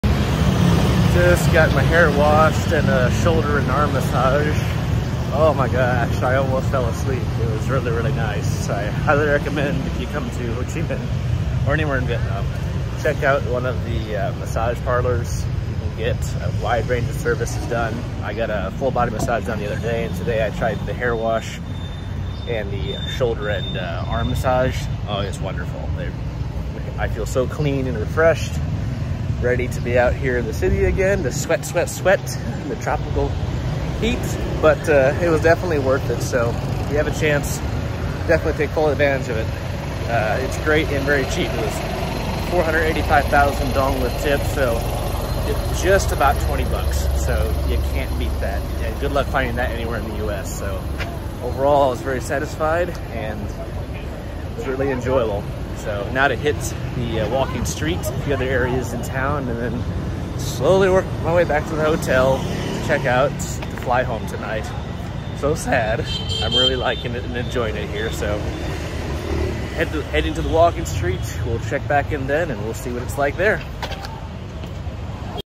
After a Massage in Ho Chi Minh City
After a relaxing head wash and massage I head back out onto the street for more sightseeing in downtown Ho Chi Mình City.